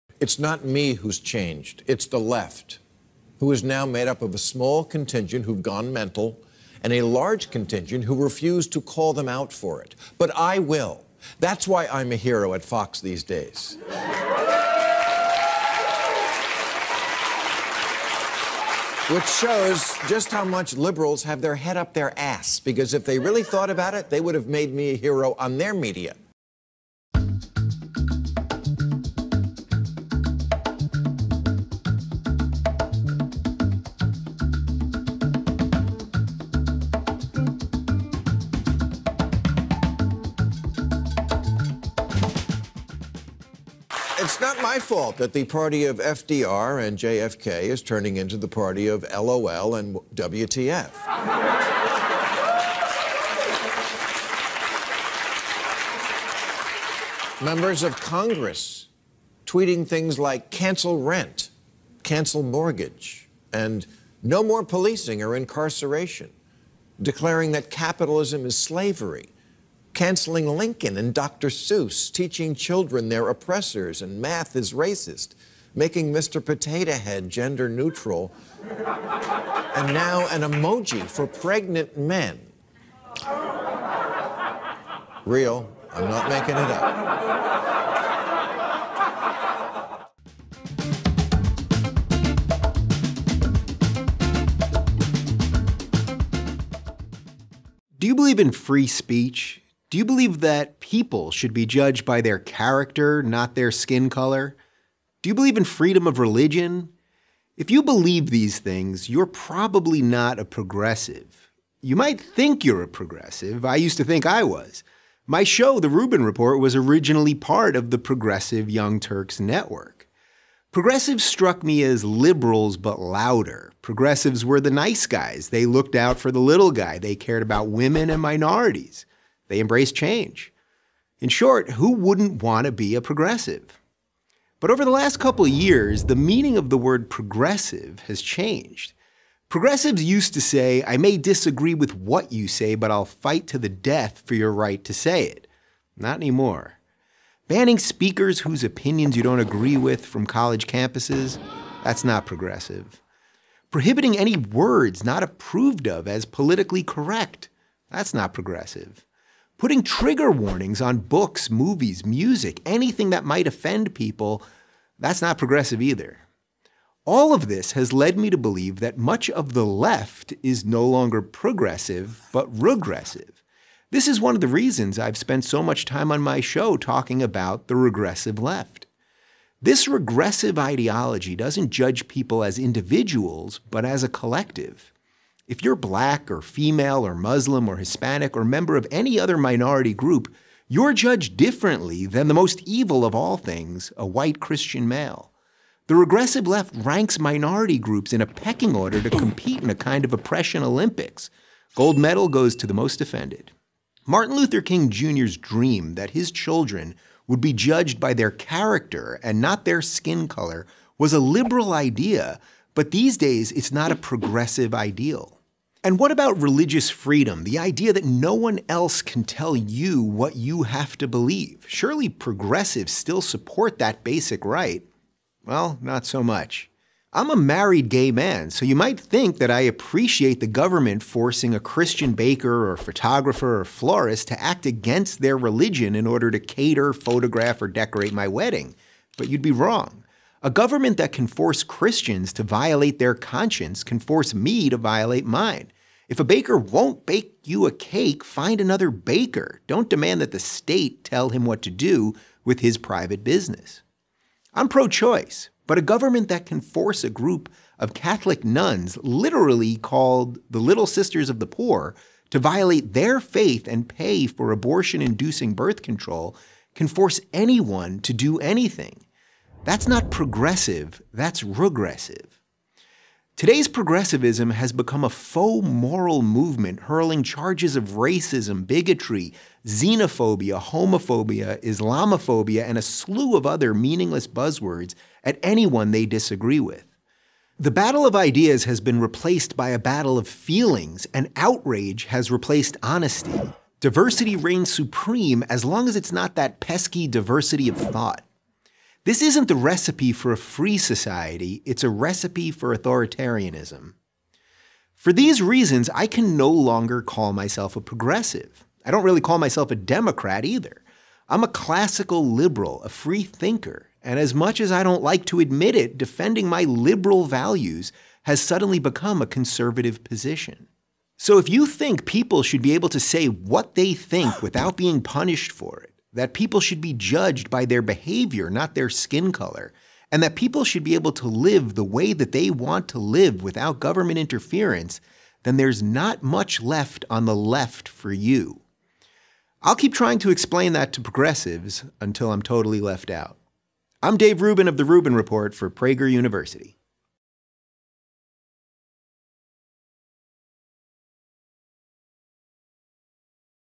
Bill Maher: I Haven’t Changed, the Left Has
Calling out the insanity of the left doesn't require abandoning liberal values—it requires staying true to them.